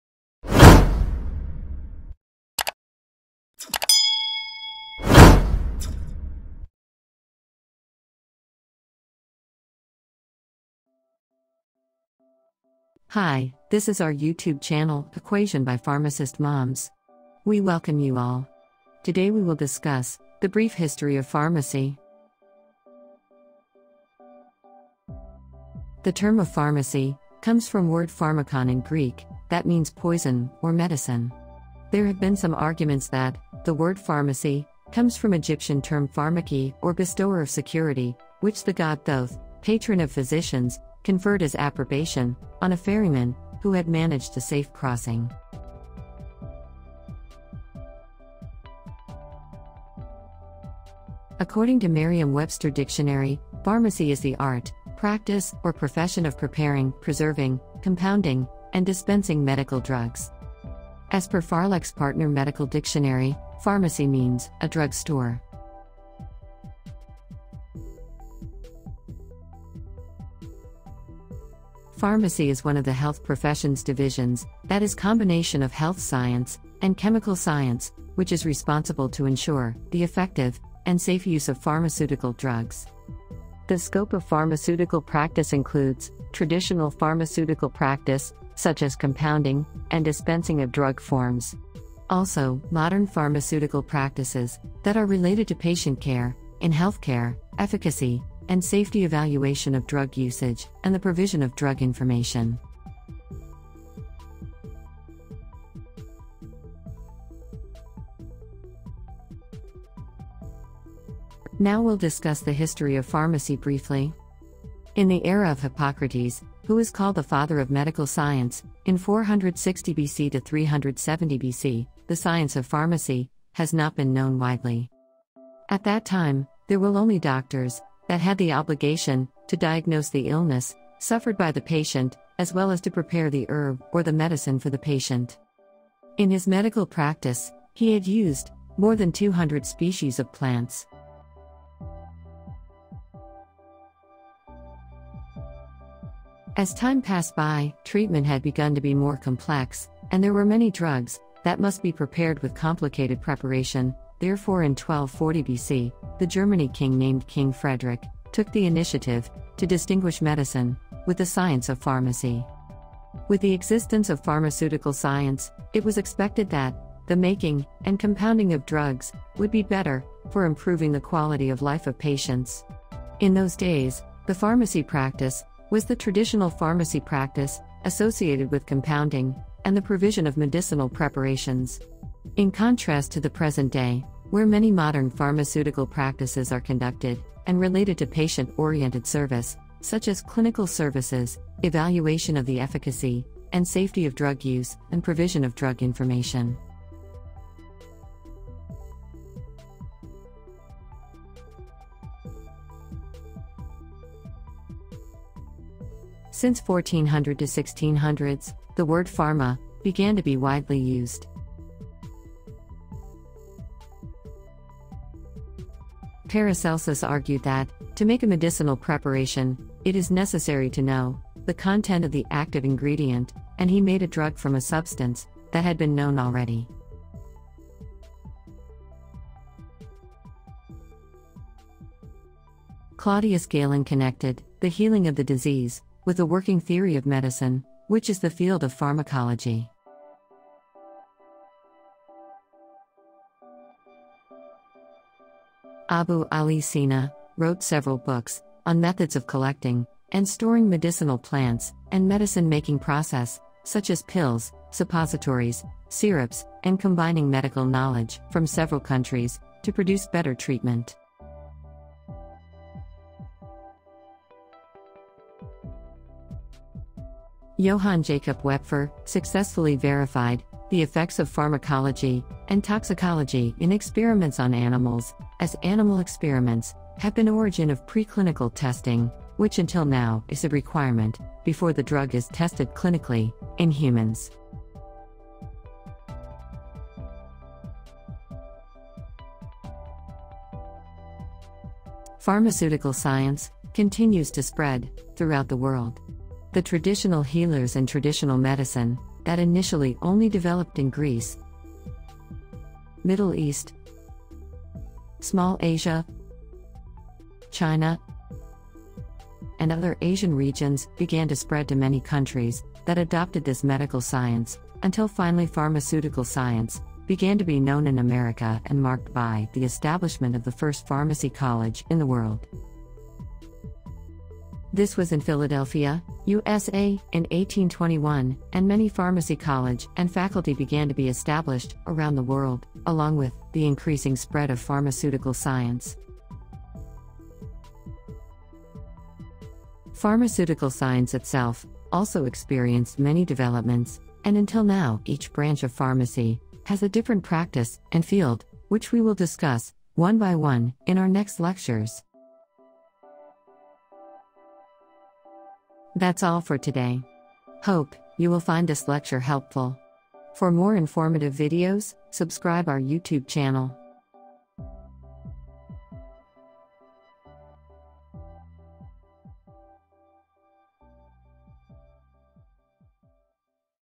Introduction to Pharmacy
This video includes the basic introduction to pharmacy, its definition, scope and brief history.